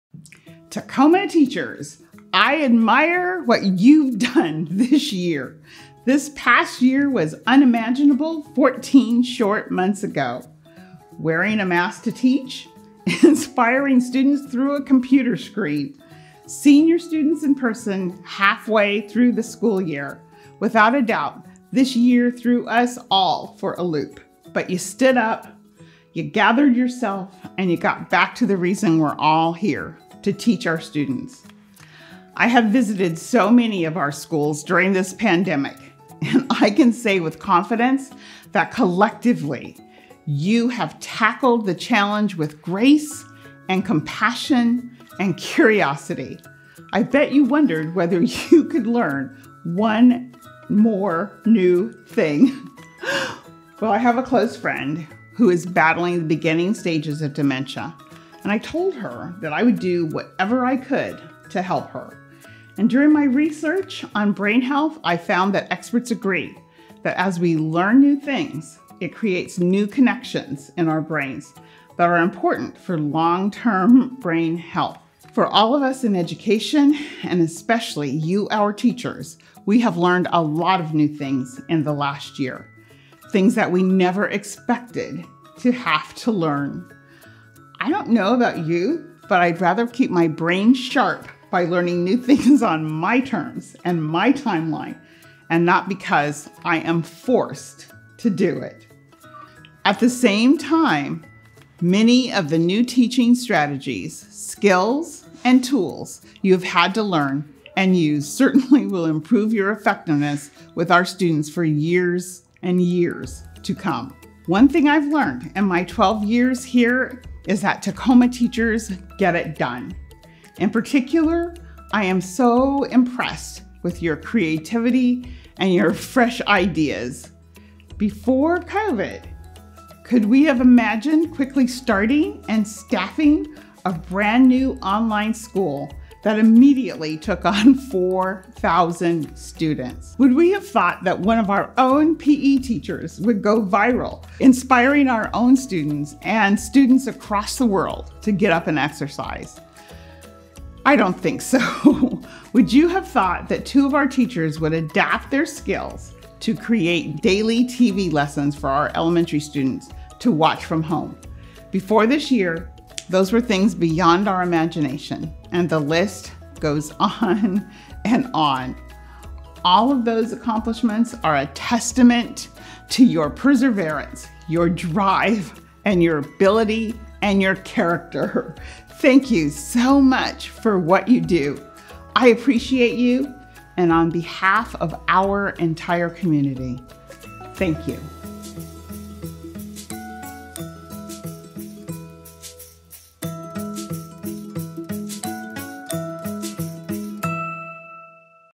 Superintendent Santorno: Thank you, teachers!
After a year no one could have imagined, Superintendent Carla Santorno is calling out some of the ways she’s seen TPS teachers go to extraordinary lengths to reach students during the pandemic.
From their fresh ideas in engaging students over a screen to their perseverance and skill, TPS teachers showed that they know how to tackle one challenge while preparing for what comes next. In this video, Superintendent Santorno thanks all TPS teachers for supporting every student, every day. Happy Teacher Appreciation Week, Tacoma teachers!